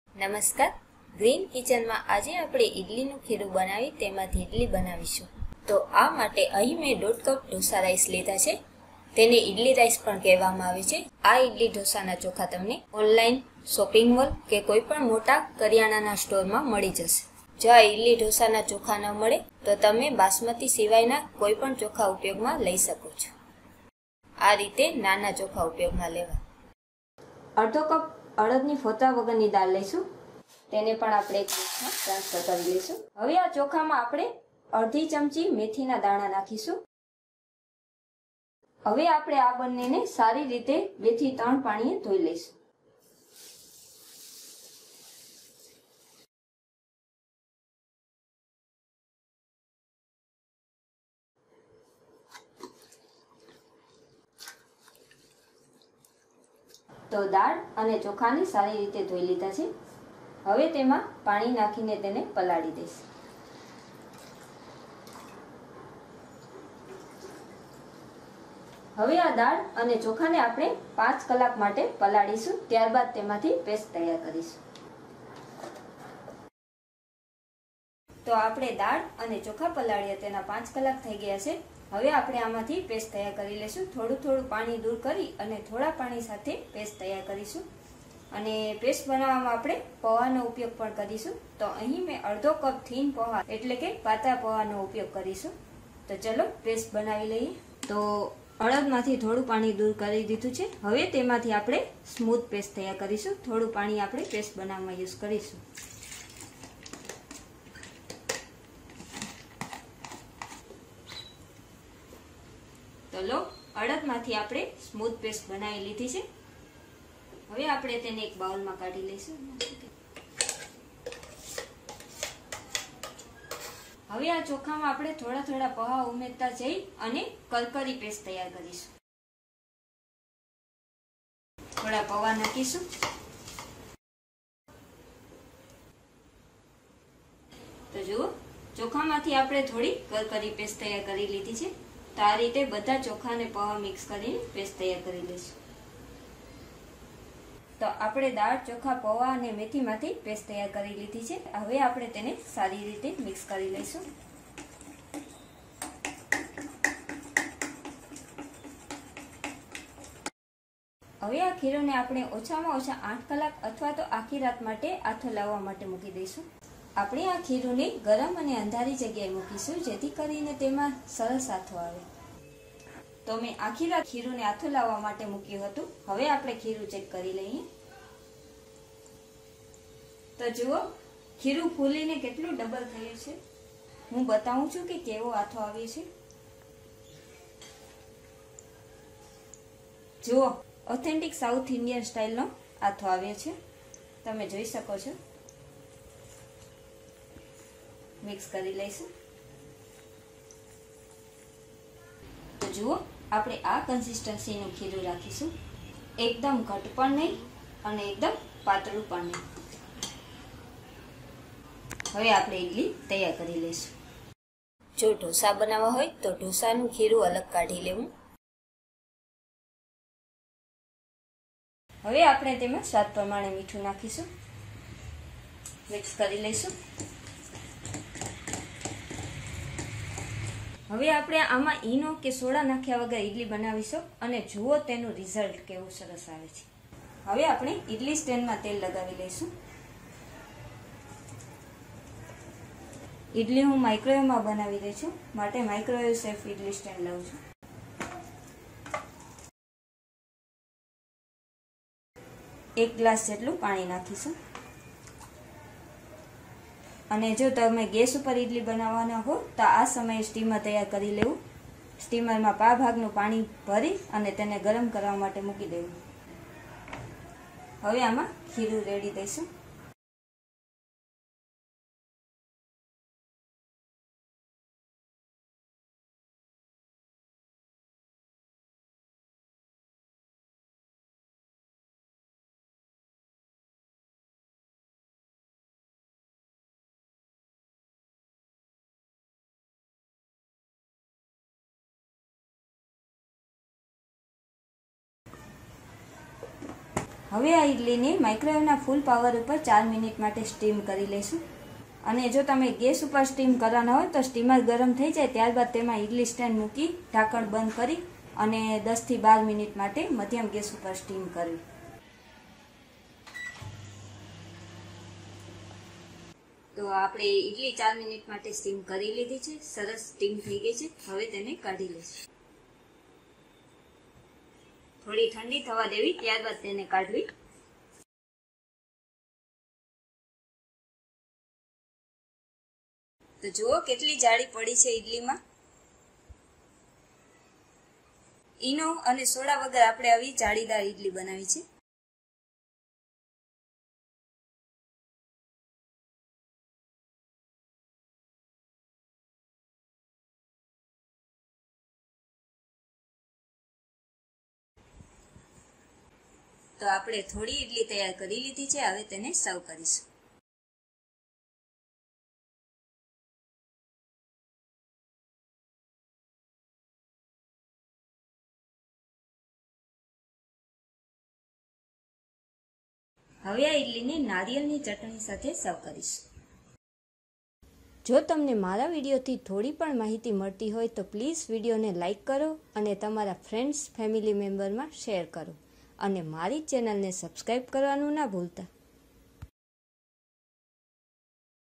ઈડલી નું ખીરું અને Super Soft Idli પોચી ઈડલી બનાવવાની રીત :Perfect Idli/Dosa Batter and Idli recipe
ઈડલી /ઢોસા નું ખીરું :Perfect Idli/Dosa Batter and Idli recipe :

સામગ્રીઃ
Idli/Dosa rice/ Boiled rice [ઈડલી / ઢોસા ના ચોખા ] અથવા
બાસમતી ચોખા સિવાયનાં કોઈ પણ ચોખા             -  1,½ કપ [ 250 થી 300 ગ્રામ ]
ફોતરાં વગર ની અડદ દાળ - ½ કપ [ 100 ગ્રામ ]
પાતળાં પૌઆ [ thin poha ] - આશરે ½ કપ
મેથી દાણા - ½ ચમચી
પાણી - જરૂર પ્રમાણે
મીઠું - સ્વાદ પ્રમાણે
તેલ - જરૂર પ્રમાણે

બનાવવાની રીત :
          નમસ્કાર Green kitchen માં આજે આપણે ઈડલી /ઢોસાનું ખીરું બનાવી તેમાંથી ઈડલી  બનાવીશું , તો આ માટે સૌ પહેલાં આપણે ઢોસાનું ખીરું બનાવી લઈશું।
          તો આ માટે મેં 1,1/2 કપ boiled rice લીધા છે , તેને ઢોસા રાઈસ પણ કહેવામાં આવે છે , ઢોસાનું ખીરું બનાવવા માટે આ ચોખાનો ઉપયોગ થાય છે , આ ચોખા તમને online , shopping mall કે કોઈપણ મોટી store માંથી મળી જશે
પણ જો આ ચોખાં ના મળે તો બાસમતી ચોખાં સિવાયનાં કોઈપણ ચોખામાંથી perfect Idli /dosa batter બનાવી શકાય, હવે 1/2 કપ અડદની ફોતરાં વગરની દાળ [ Urad  dal ] લઈશું , તેને એક બીજા વાસણમાં લઈશું , હવે આ ચોખામાં 1/2 ચમચી મેથીનાં દાણા નાખીશું , તેનાથી ઢોસા નો co-lour પણ સારો આવશે અને આથો પણ સારો આવશે , હવે આપણે આ બન્ને ને બે થી ત્રણ પાણી એ ધોઈ લઈશું , તો દાળ અને ચોખાને ધોઈ લીધા છે હવે તેમાં પાણી નાખીને પલાળી દઈશું , દાળ અને ચોખાને 5 કલાક પલાળીશું , 5 કલાક થઇ ગયા છે હવે આમાંથી paste બનાવીશું થોડું પાણી દૂર કરી થોડા પાણી સાથે paste બનાવીશું , paste બનાવવા આપણે પૌઆ નો ઉપયોગ કરીશું ,અહીં મેં 1/2 કપ thin poha [ પાતળા પૌઆ ] લીધા છે , હવે અડદ માંથી આપણે smooth સ્મૂથ પેસ્ટ બનાવીશું , અને ચોખામાં પૌઆ નાખતા જઈ થોડી કરકરી પેસ્ટ બનાવીશું , તો આપણે દાળ , ચોખા, પૌઆ અને મેથી  માંથી પેસ્ટ બનાવી તેને એક બાઉલ માં લઇ લીધી છે , હવે આપણે તેને સારી રીતે mix કરી લઈશું , હવે આ ખીરુંને ઢાંકીને ઓછામાં ઓછા 8 કલાક અથવા આખી રાત આથો લાવવા મૂકી દઈશું , આપણે આ ખીરુંને ગરમ અને અંધારી જગ્યાએ મુકીશું જેથી તેમાં સરસ આથો આવે , તો જુઓ આપણે ખીરુંને આખી રાત આથો લાવવા મૂક્યું હતુંને કેવો સરસ આથો આવ્યો , ખીરું ફૂલીને double થઇ ગયું છે , હવે આપણે આ બધા ખીરુંમાં અથવા ઢોસા બનાવવા હોય તેટલું ખીરું અલગ લઇ તેમાં સ્વાદપ્રમાણે મીઠું [ salt ] નાખીશું , ત્યારબાદ idli stand ને તેલ વડે ચીકણું કરીશું , હું microwave safe ઈડલી stand લઉં છું, એમાં ઇડલીનું ખીરું રેળવું , હવે એક ગ્લાસ પાણી નાખી microwave ના full power માં 4 મિનીટ માટે steam કરીશું , જો ગેસ ઉપર ઈડલી બનાવવી હોય તો ઇડલિયું નું પાણી ગરમ થાય ત્યાર બાદ stand મૂકી , ઢાંકીને 10 થી 12 મિનીટ steam કરવી , ઈડલી થોડી ઠંડી થાય ત્યારે stand માંથી કાઢવી ,જુઓ સરસ Soft પોચી ,જાળીદાર ઈડલી તૈયાર છે , નારિયેર ની ચટણી સાથે serve કરીશું।

Here you will find Gujarati recipes in Gujarati language.